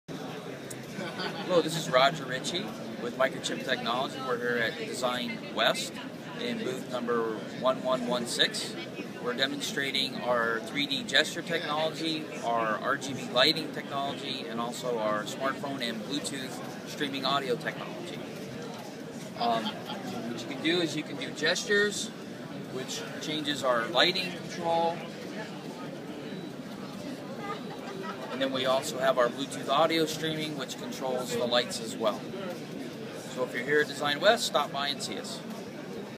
Microchip at designwest booth 1116
Microchip Technology  Booth #1116 Rodger Richey Director of applications  Discussing 3D position tracking and gesture recognition, Intelligent lighting and control, and USB & Bluetooth audio docking.  DesignWest 2013.